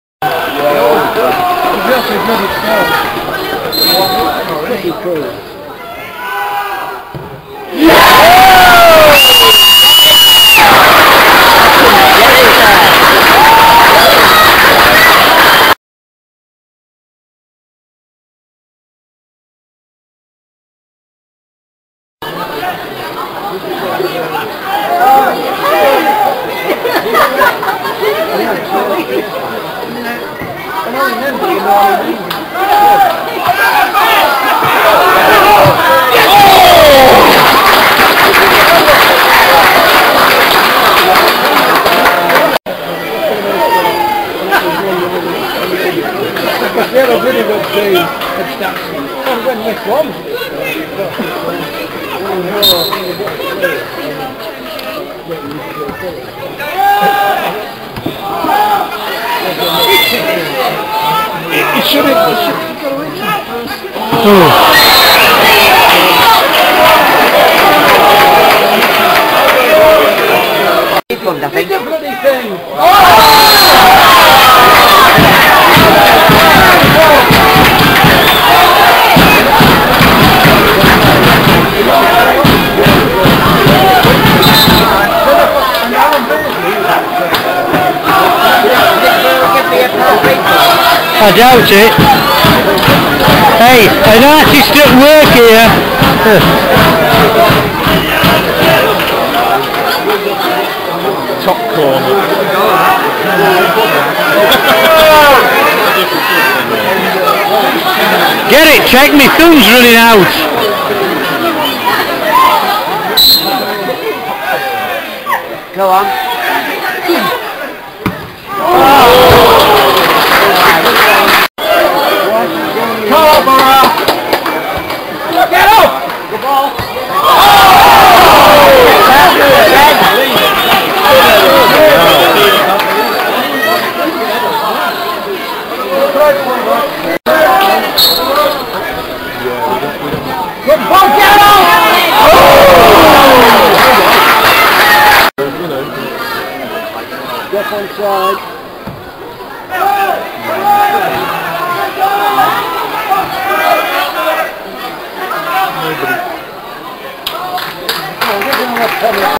BORO V BARROW 2006
Night time action from the McCain Stadium as Scarborough F.C. meet Barrow in a Conference North fixture on November 14th 2006